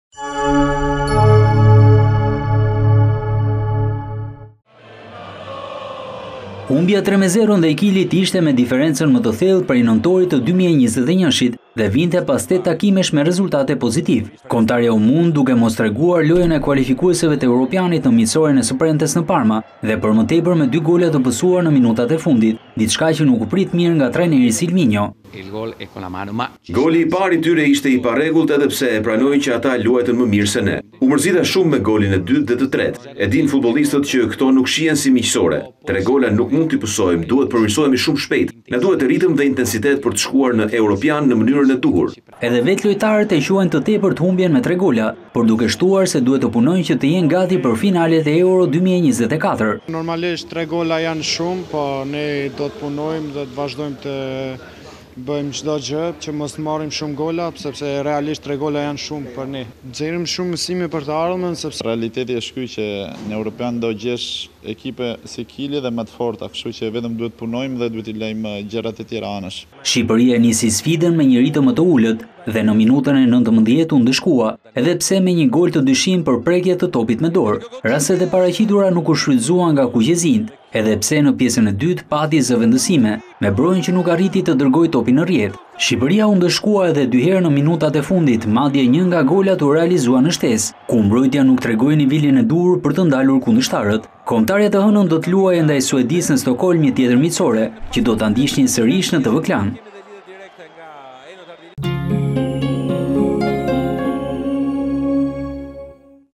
Tv Klan - Sylvinho pas humbjes: Nuk duhej të pësonim 3 gola
Humbja 3 – 0 ndaj Kilit ishte me diferencën më të thellë prej Nëntorit të 2021 dhe vinte pas 8 takimesh me rezultate pozitive.

Kombëtarja u mund duke mos treguar lojën e kualifikueseve të Europianit në miqësoren së premtes në Parma dhe për më tepër me 2 gola të pësuar në minutat e fundit, diçka që nuk prit mirë nga trajneri Sylvinho.

“Goli i parë i tyre ishte i parregullt edhe pse e pranoj që ata luajtën më mirë se ne. U mërzita shumë me golin e dytë dhe të tretë. E dinë futbollistët që këto nuk shikohen si miqësore. 3 gola nuk mund t’i pësojmë, duhet të përmirësohemi shumë shpejt. Na duhet ritëm dhe intensitet për të shkuar në Europian në mënyrën e duhur”.

Edhe vetë lojtarët e quajnë të tepërt humbjen me 3 gola, por duke shtuar se duhet të punojnë që të jenë gati për finalet e Euro 2024.

“3 gola janë shumë, por ne duhet të punojmë që mos të pësojmë 3 gola. Të nxjerrim mësime për të ardhmen”, tha Ramadani.

“Realiteti është ky në Europian do të kemi ekipe si Kili dhe më të forta dhe duhet të punojmë e t’i lemë gjërat e tjera pas”, u shpreh Rei Manaj.

Shqipëria e nisi sfdën me një ritëm më të ulët dhe në minutën e 19’ u ndëshkua edhe pse me një gol të dyshimtë për një prekje të topit me dorë. Rastet e paraqituar nuk u shfrytëzuan nga kuqezinjtë edhe pse në pjesën e dytë pati edhe zëvendësime me Brojën që nuk arriti të dërgojë topin në rrjetë.

Shqipëria u ndëshkua edhe dy herë në minutat e fundit madje një nga golat u realizua në shtesë, ku mbrojtja nuk tregoi nivelin e duhur për të ndalur kundërshtarët.

Kombëtarja të hënën do të luajë ndaj Suedisë në Stokholm, një tjetër miqësore që do ta ndiqni sërish në Tv Klan.